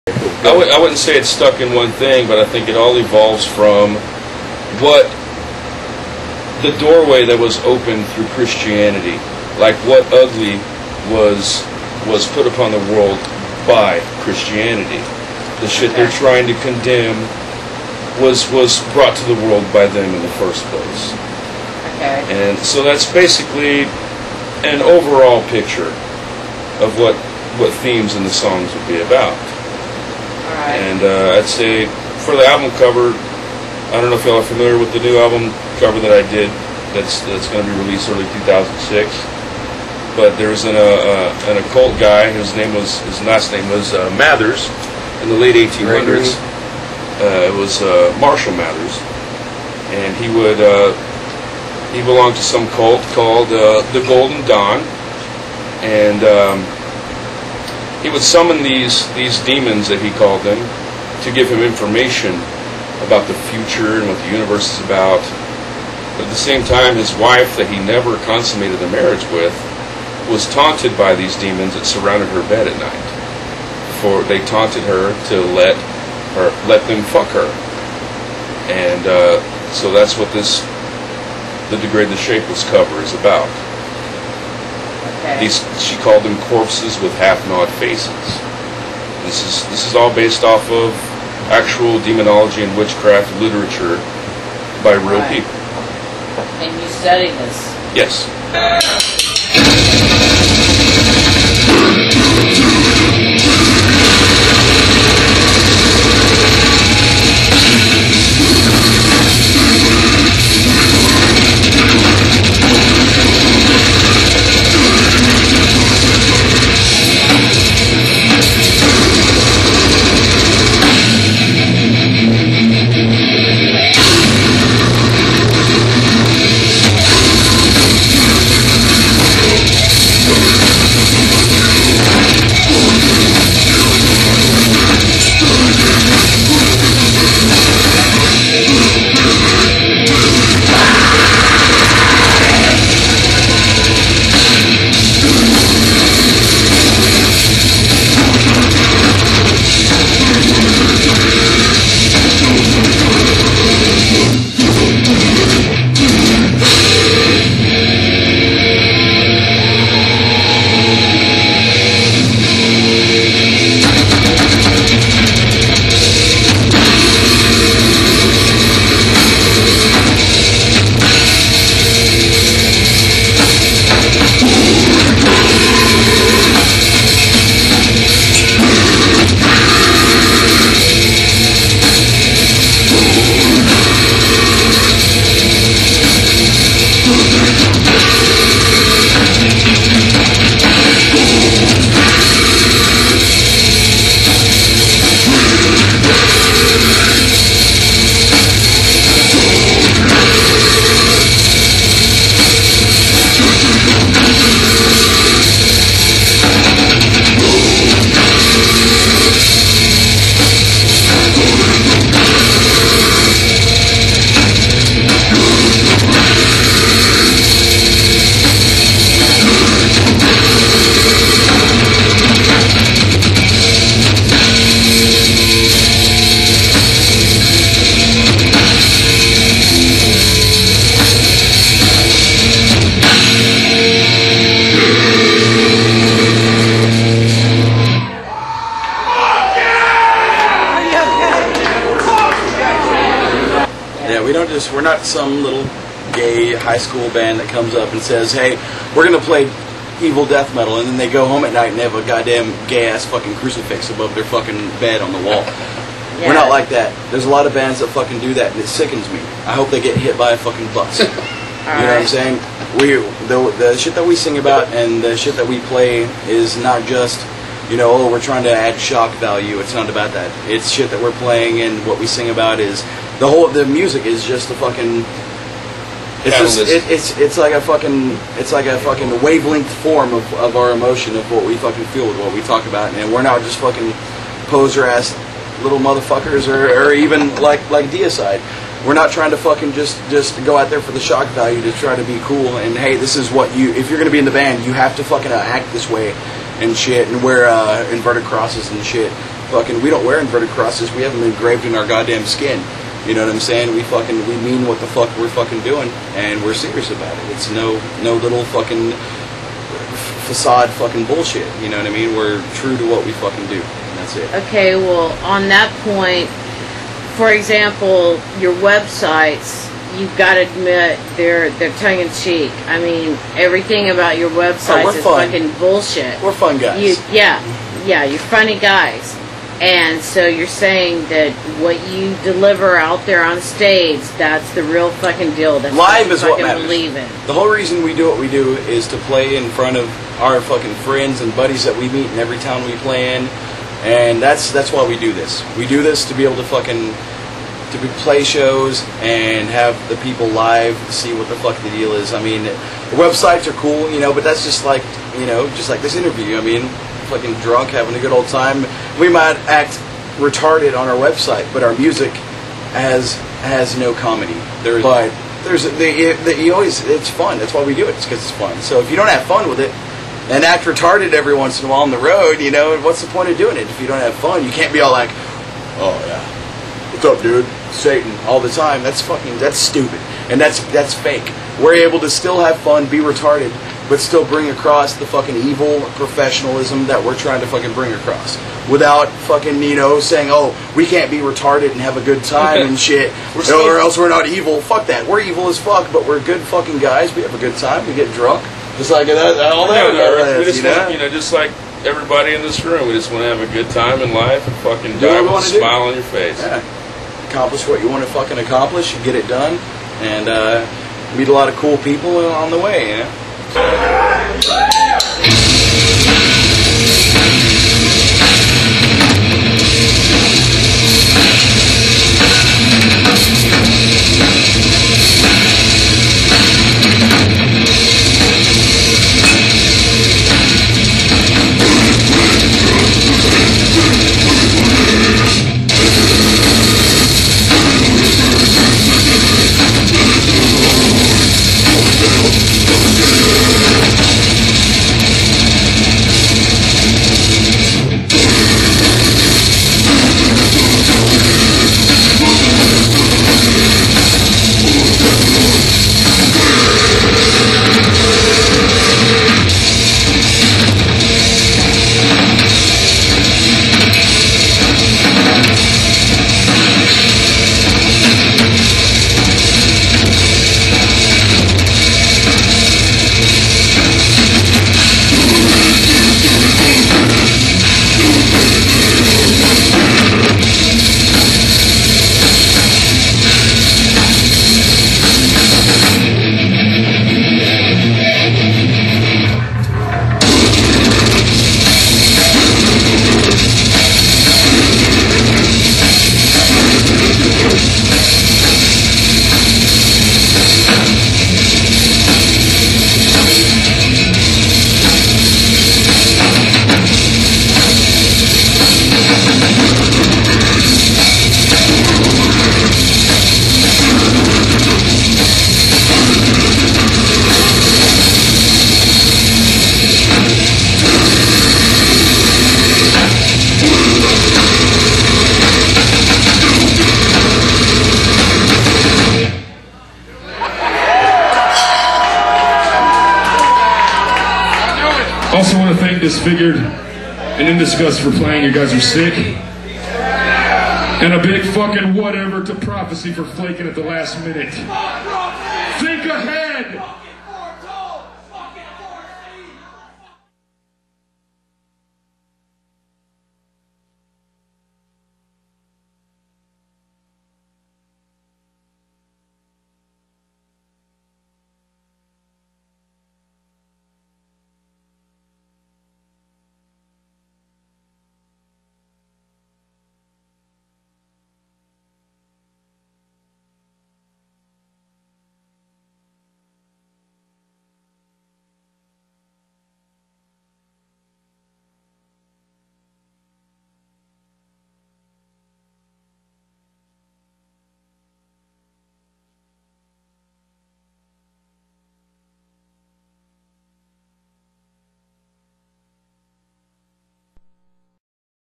Pure Metal Sickness Presents Images of Violence pt 2 of 2
Pure Metal Sickness presents Images of Violence with Tammy Moore interviewing and live show footage produced by Jennifer "Evil Carrots" Henneck.